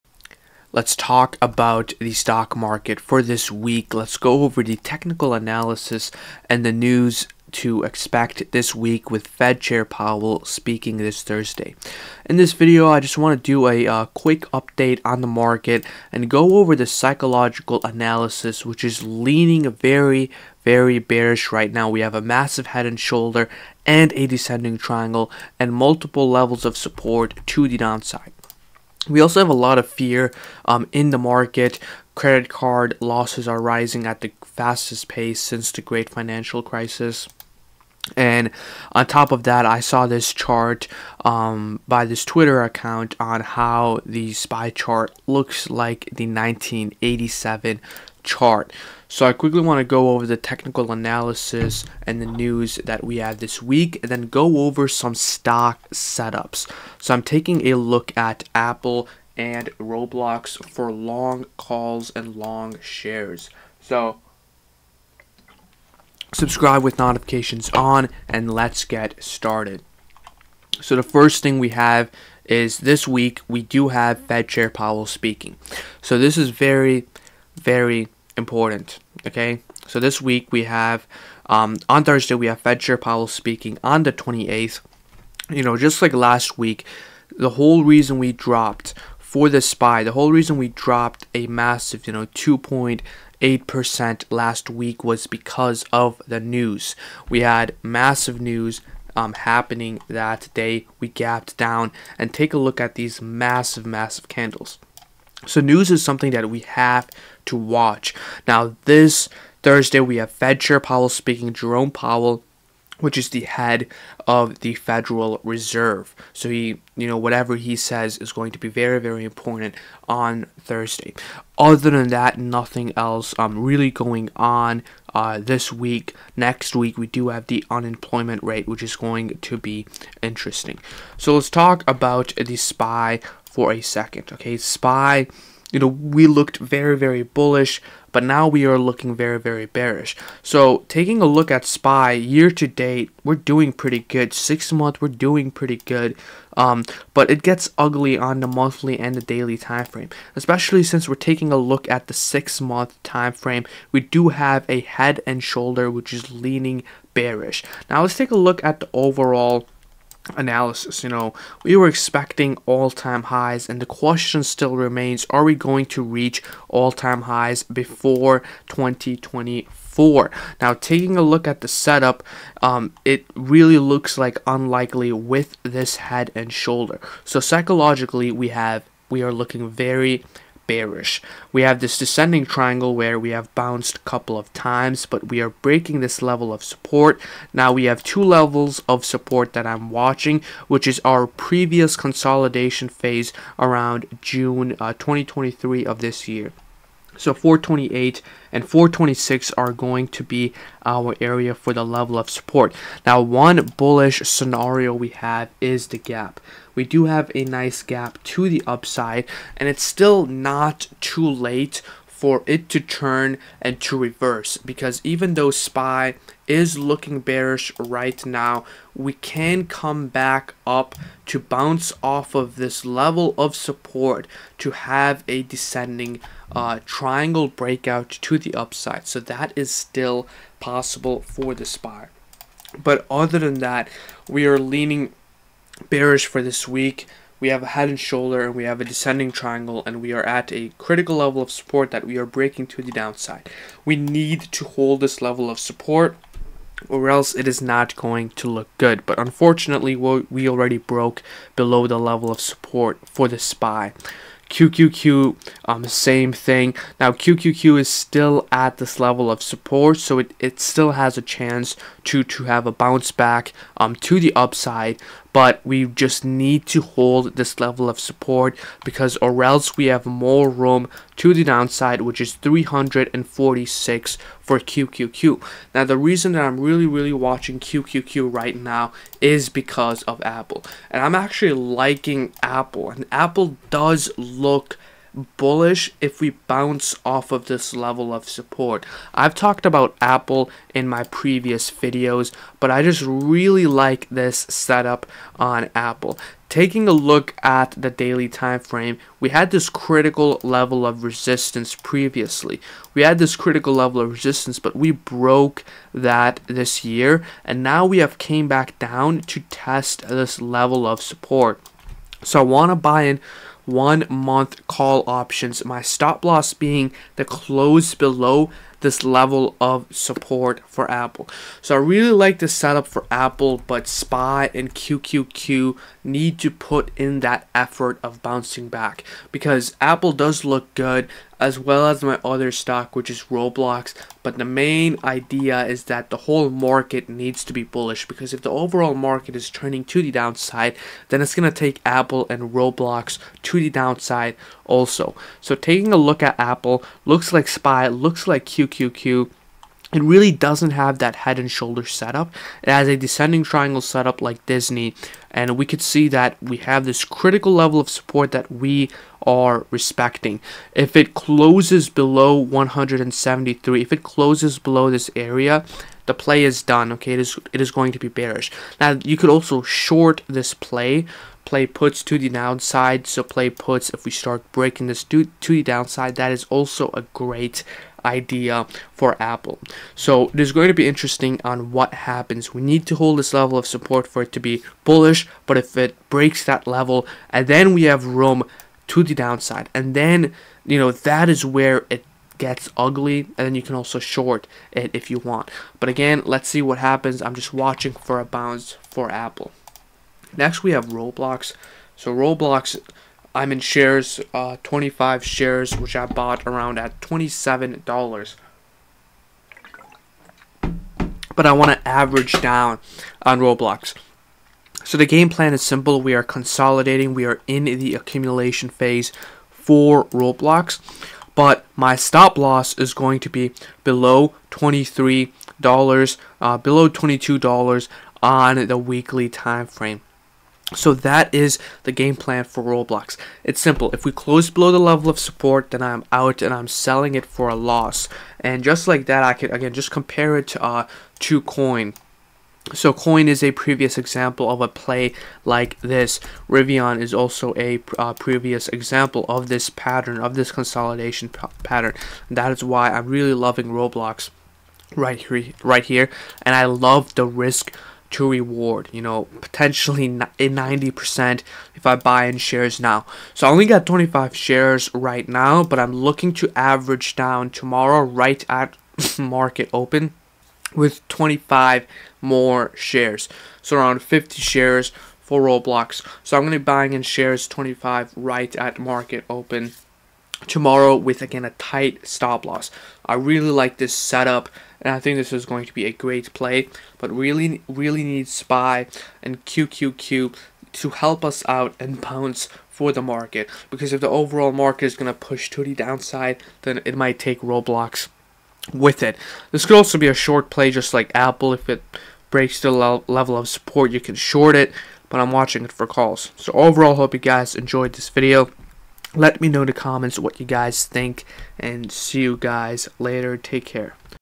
If 1987 Is To Happen Again, Next Week Should Be It!
— 💡[FREE STOCKS on MOOMOO] [Open an account and deposit $1]

0:00 - Intro
1:11 - Fed Chair Powell Speaking
2:11 - SPY
4:14 - QQQ
4:40 - AAPL
6:42 - RBLX